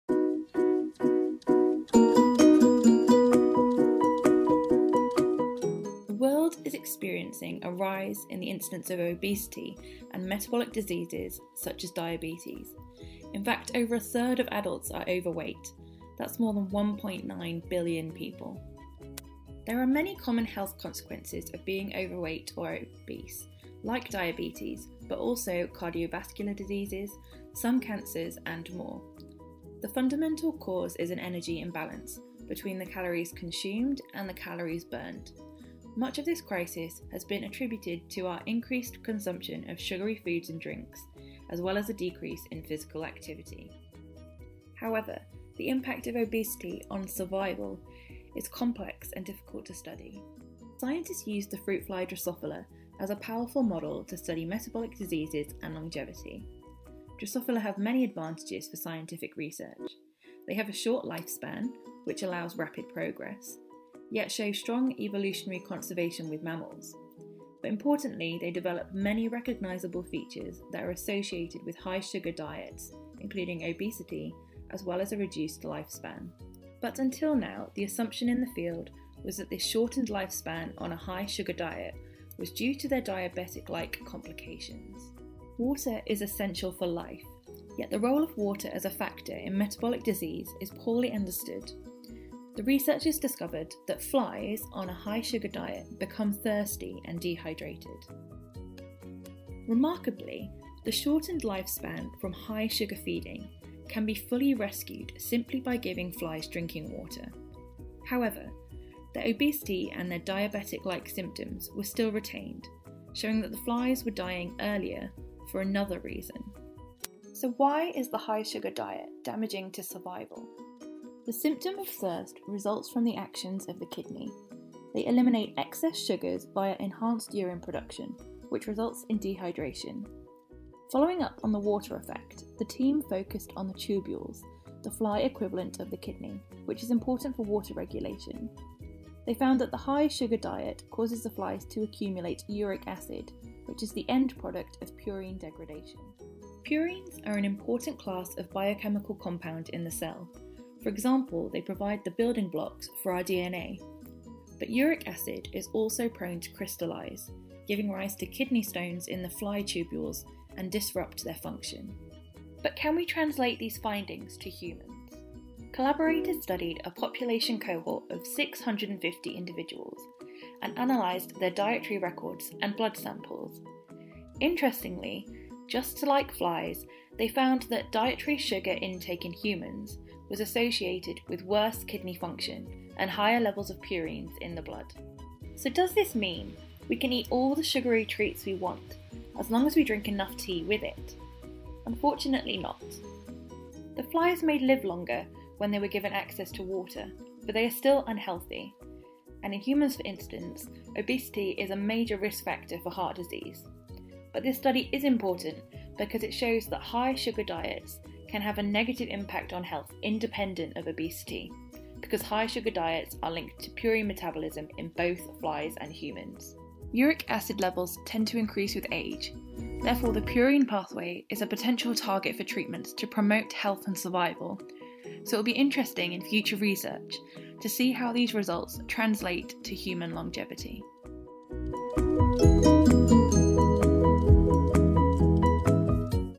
Sugar leads to an early death, but not due to obesity
New research from our Redox Metabolism group reveals how a high sugar diet leads to an early death, but not due to obesity as we had assumed. Find out what our colleagues latest research tells us instead in this video.

Animation software used: Videoscribe